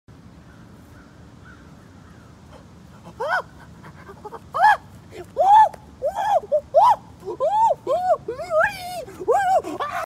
WHAT IS THIS THING?
Here, we see a young primate discovering an energy portal of some sort. GO FIGURE!